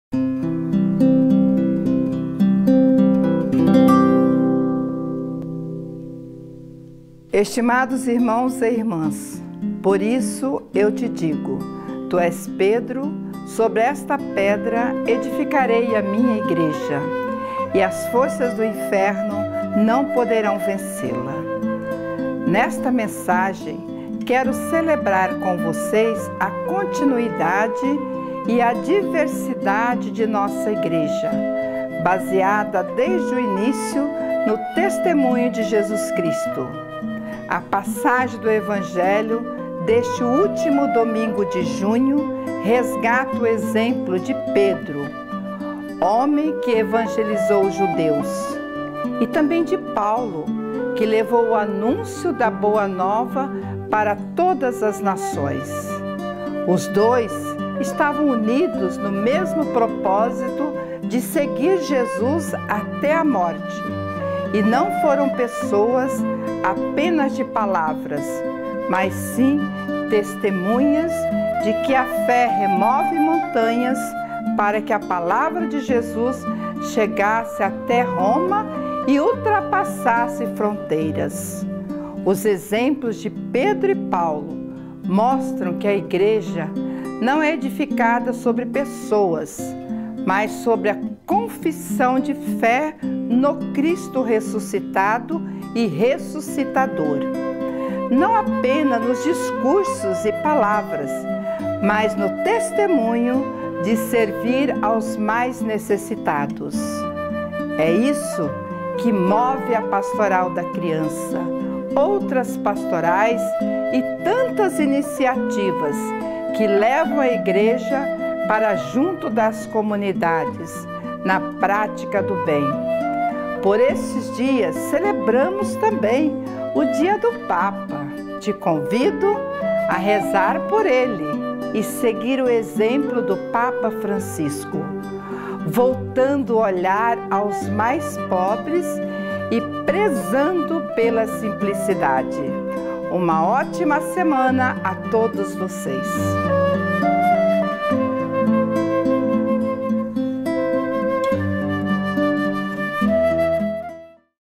A verdadeira fé se mostra em atos - Vídeo mensagem Ir. Vera Lúcia (28/06/2015)
No vídeo dessa semana, Ir. Vera lembra que, a exemplo de Paulo e Pedro, nossa fé deve ser edificada no nosso testemunho de servir a Deus através do que Cristo ensinou: não apenas por palavras e discursos, mas por atos.
Este vídeo foi produzido com a colaboração técnica da Associação Evangelizar é Preciso.